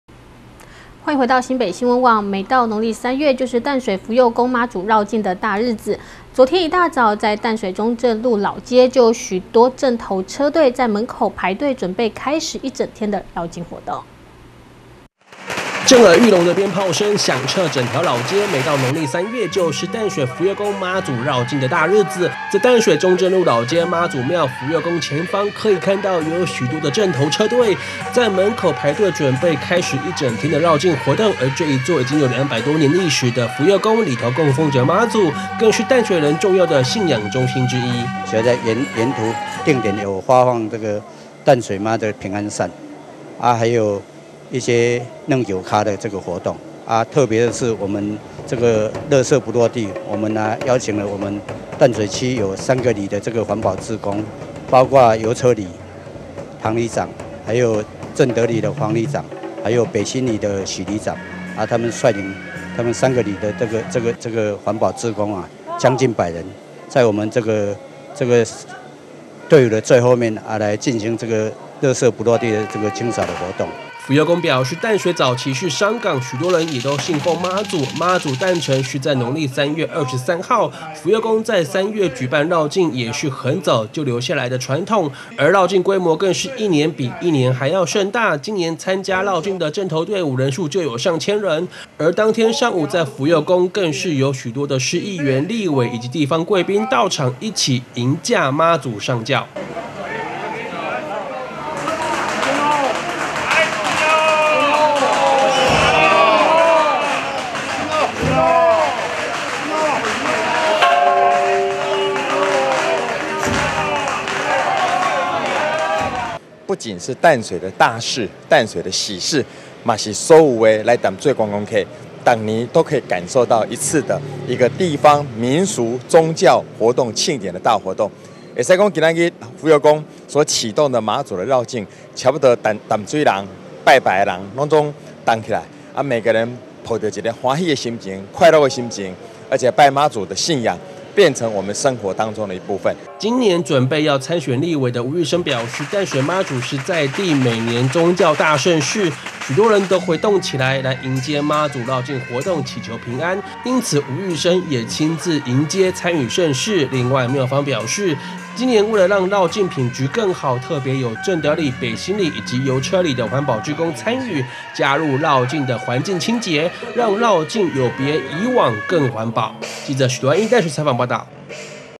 福佑宮媽祖遶境 眾人迎駕老街熱鬧
震耳欲聾的鞭炮聲，響徹整條老街，每到農曆三月，就是淡水福佑宮媽祖遶境的大日子，因此昨天一大早，在淡水中正路老街媽祖廟福佑宮前方，可以看到有許多的陣頭車隊，在門口排隊準備開始一整天的遶境，而這座已經有兩百多年歷史的福佑宮，裡頭供奉著媽祖，更是淡水人重要的信仰中心之一。

淡水福佑宮主委 謝安棋

淡水福佑宮表非示，淡水早期是商港，許多人也都是信奉媽祖，媽祖誕辰是在農曆的三月二十三，福佑宮在三月舉辦遶境也是很早就留下來的傳統，而遶境的規模更是一年比一年盛大，今年參與遶境的陣頭隊伍人數有上千人，而當天上午在福佑客更有許多市議員，立委及地方貴賓到場一起迎駕媽祖上轎。

立委擬參選人 吳育昇

今年準備要參選立委的吳育昇表示，淡水媽祖在地上是，是每年的宗教大盛事，許多人都會動起來，迎接媽祖繞境活動，祈求平安，因此吳育昇也親自迎接參與盛事，另外廟方表示，今年為了讓繞境品質更好，特別有正德里、北新里以及油車里的環保志工參與，加入繞境的環境清潔，讓繞境有別於以往更環保。